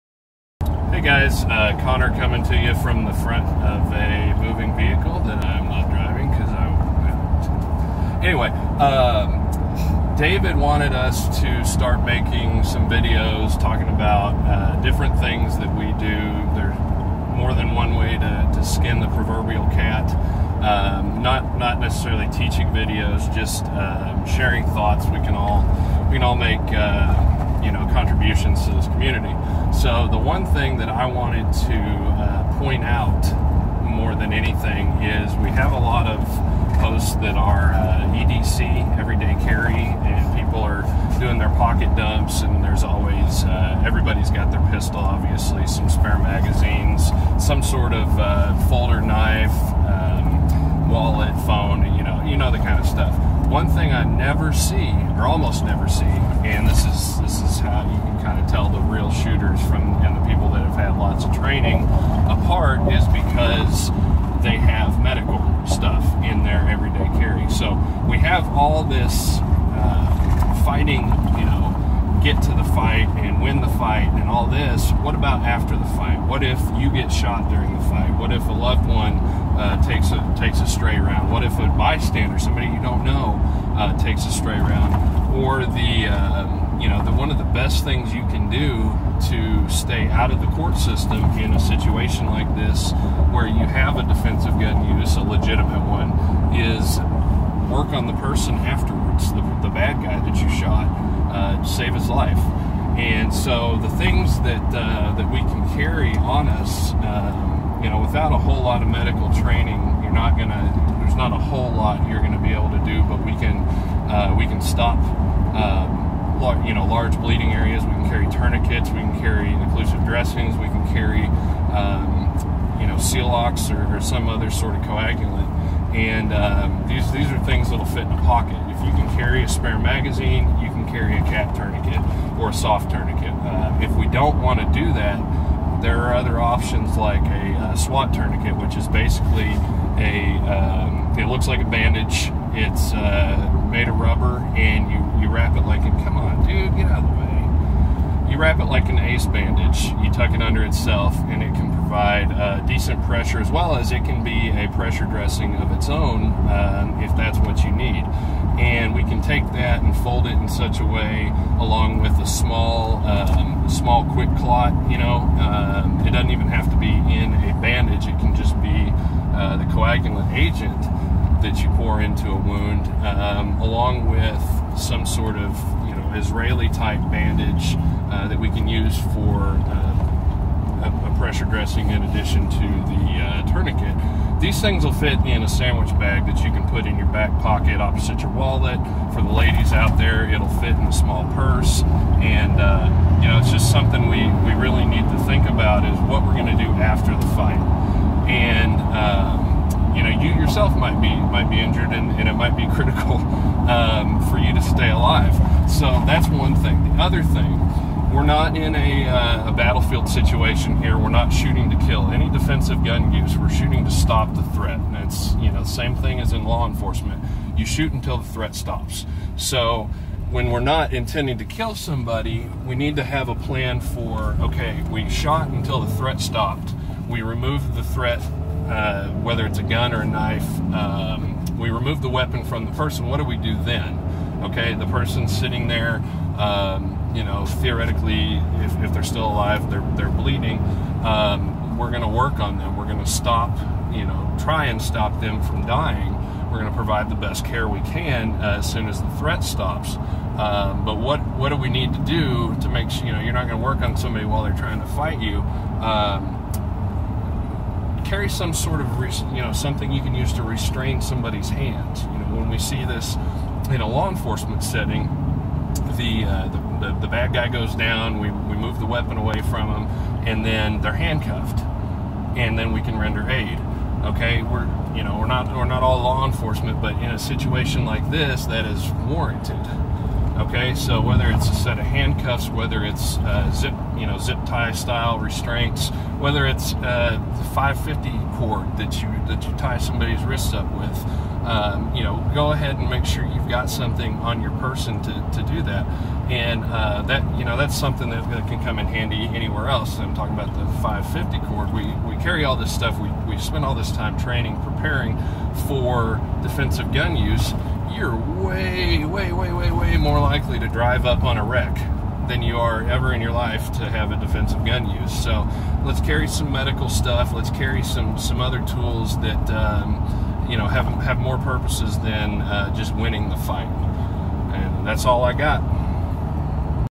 EDC Components we *Should* be Carrying
In addition to our daily supplies and force multipliers,  what else should we carry? Here are some of my thoughts on the matter.